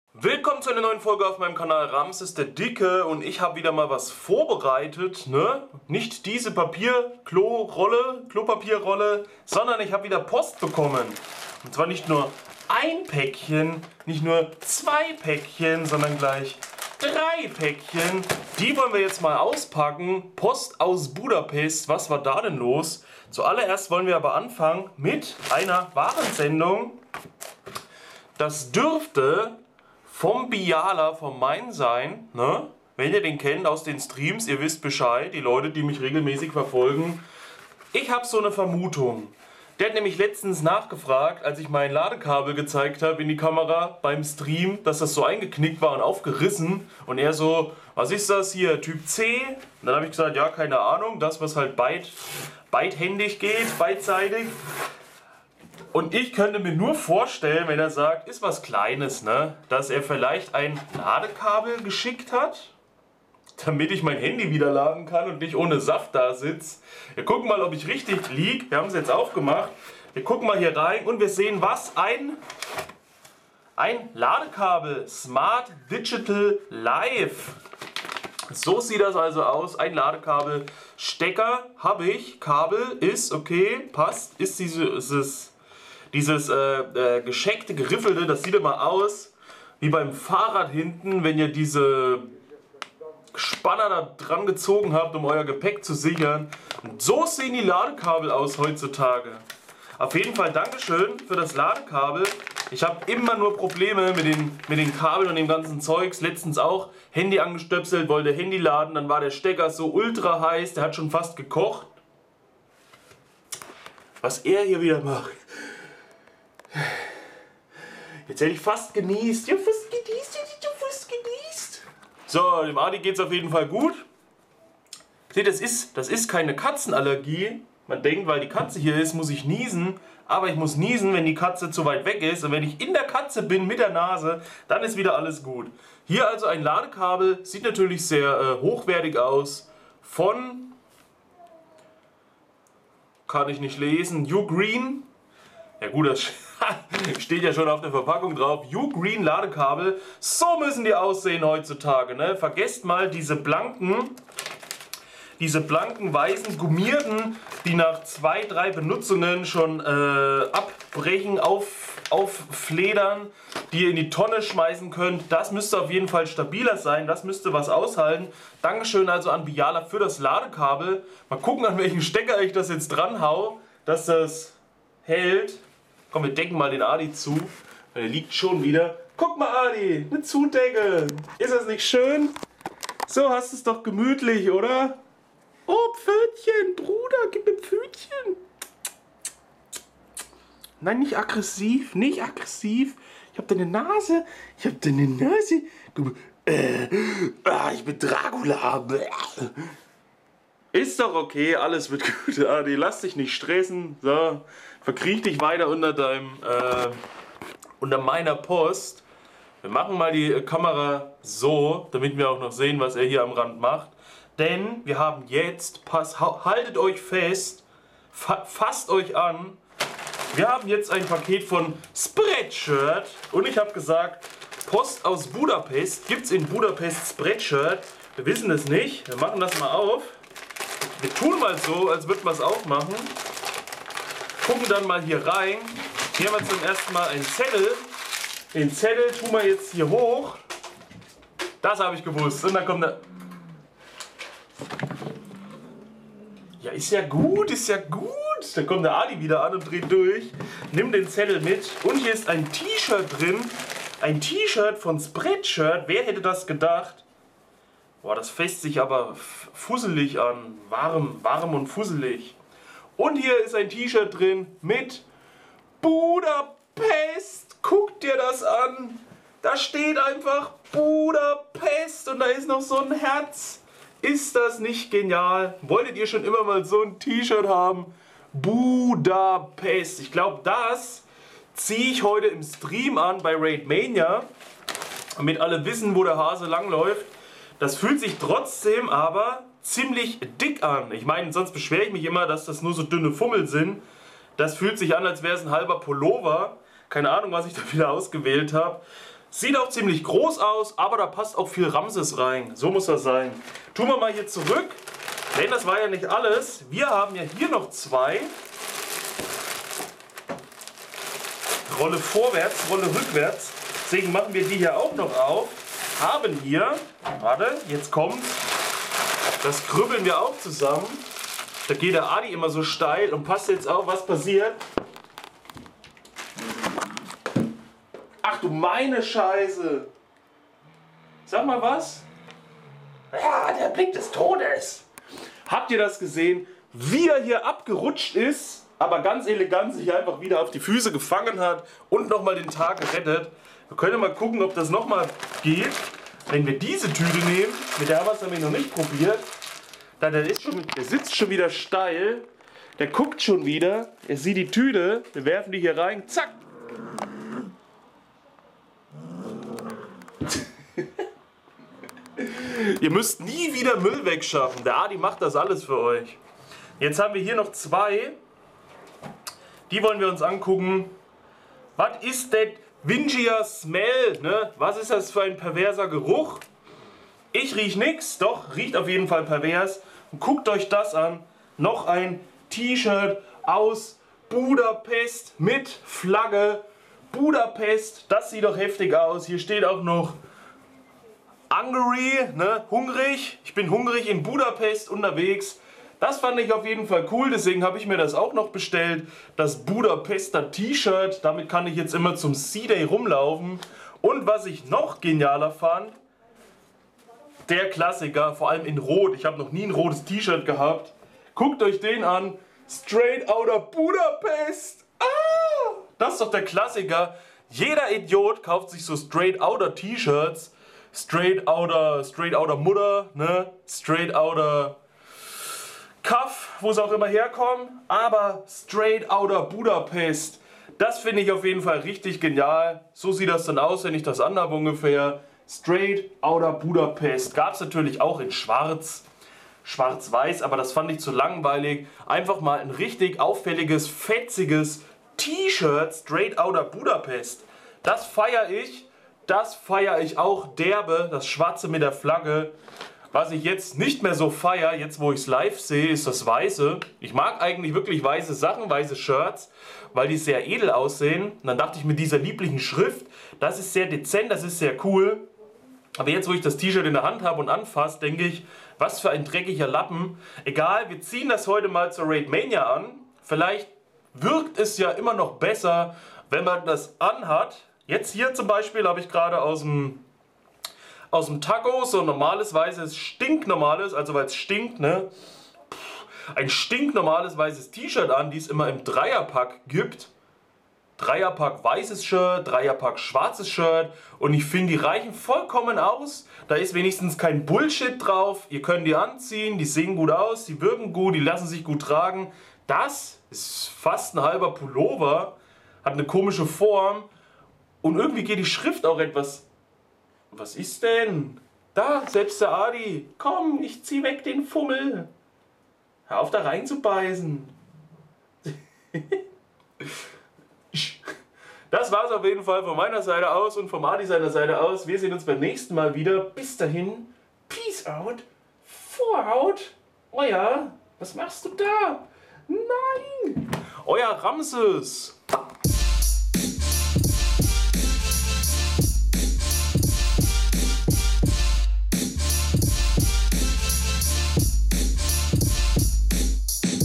[UNBOXING] | Post aus Budapest & Biala
Heute gibts die exklusive Budapest Edition :D

Da ich die 199 meistens voll habe kann es eine Weile dauern bis man eine Chance hat in die Liste zu kommen.

Viel Spaß